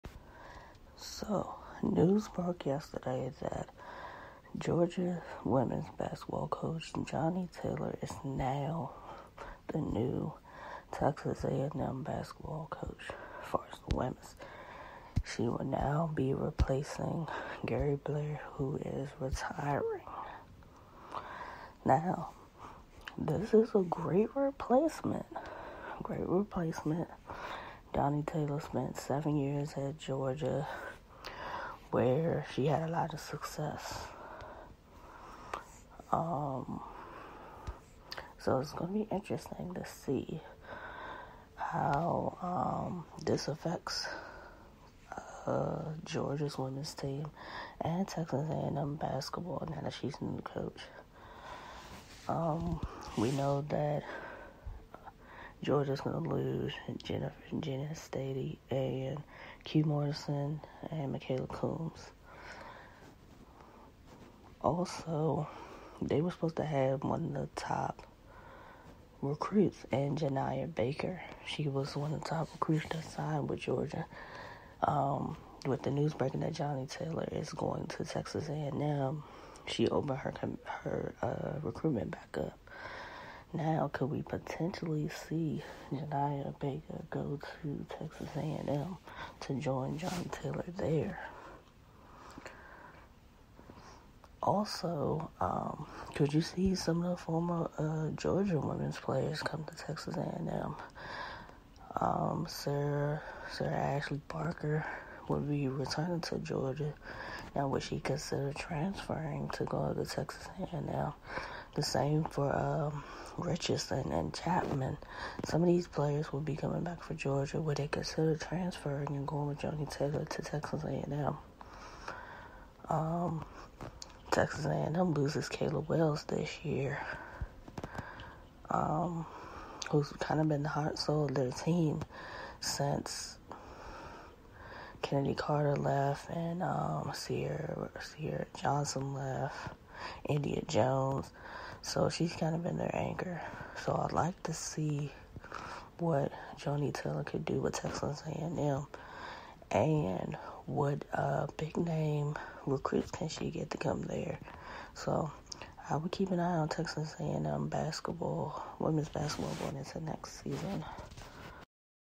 Joni Taylor the new Texas A&M WBB coach
Joni Taylor is the new Texas A&M WBB coach. She replaces Gary Blair as he is retiring.